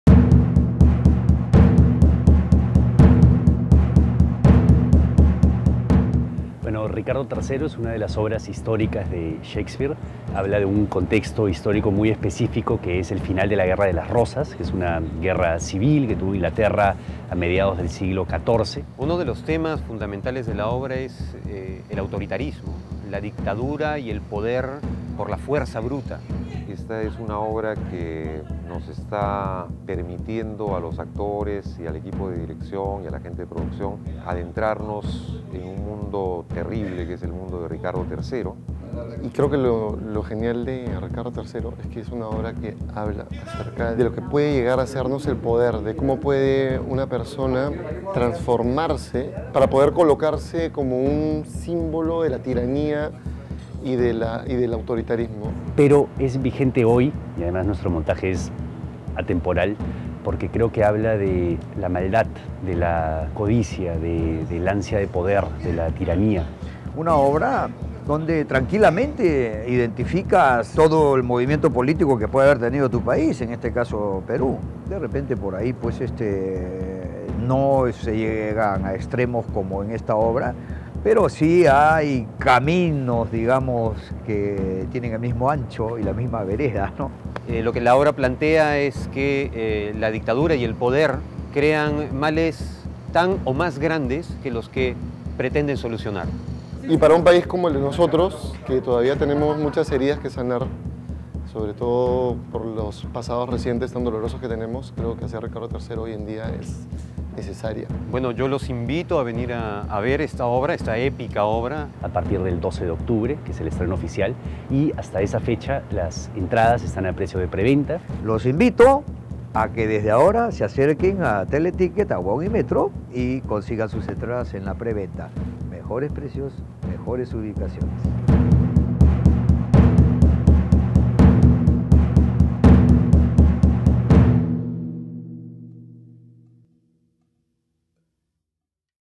Contexto Histórico de Ricardo III
Ricardo está decidido a ser rey de Inglaterra. Su plan asombra y aterroriza. Hambriento de poder hará lo que sea para llegar al trono pero necesitará de algunos aliados que irá consiguiendo gracias a su peligroso poder de seducción. Su cómplice principal somos nosotros, su público.